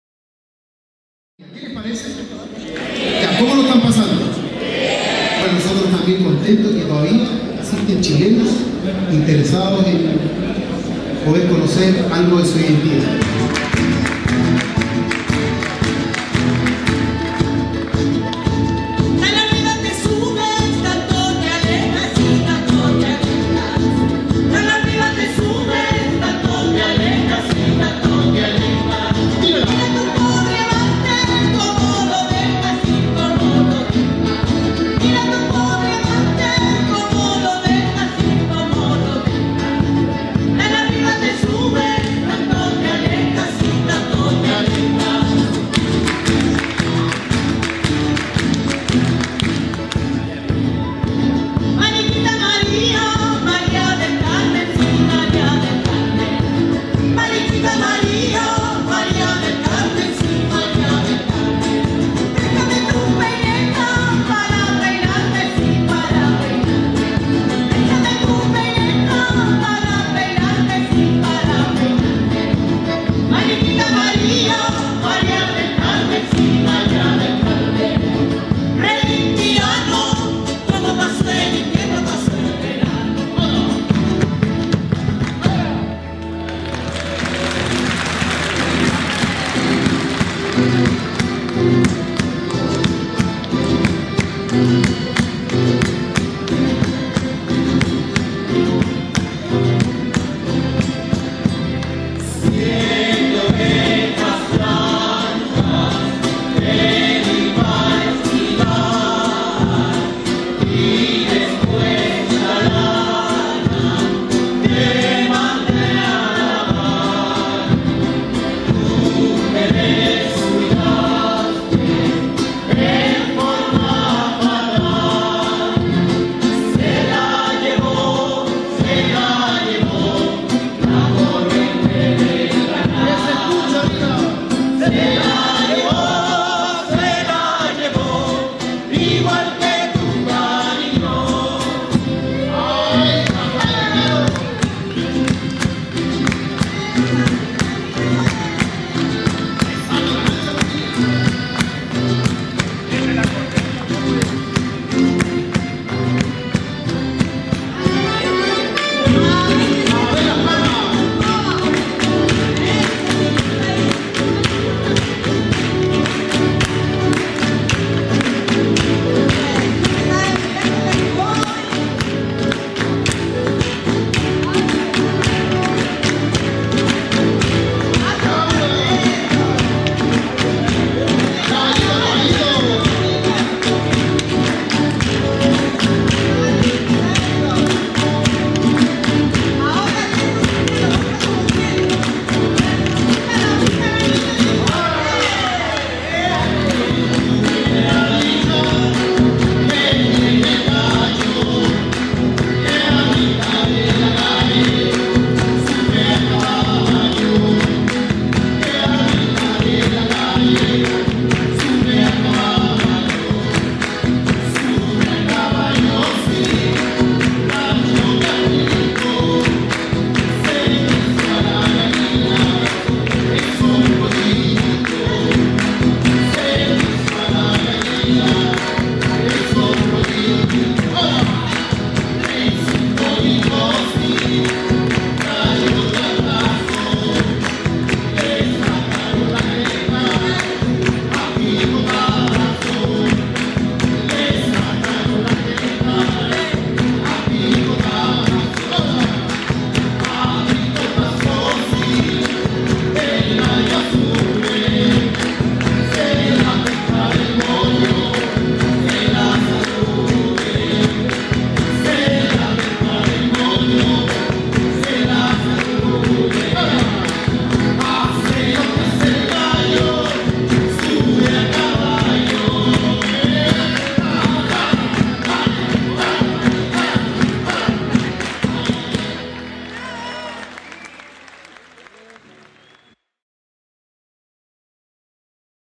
Taller Calahuala.avi
En el marco del bicentenario celebramos junto al Museo Histórico Nacional, la muestra, Artesanía Canto y Danza 2010, la que convoco a 27 expositores todos artesanos en diferentes rubros. La exposición fue realizada en el patio colonial del Museo Histórico Nacional y estuvo acompañada por los cantos y danzas de los conjuntos folklóricos asistentes, los que dejaron en alto relieve nuestra cultura música y creencias populares. En este video hemos seleccionado algunas tomas y canciones que son parte  del trabajo realizado por el Taller Calahuala lo que consideramos un valioso aporte a nuestra Cultura Nacional. El evento fue Producido por el Sr. Alejandro Hermosilla Vásquez y Alberto Ensignia (Taller Alechvaz) y el patrocinio del Museo Histórico Nacional,        Sr. Leonardo Mellado.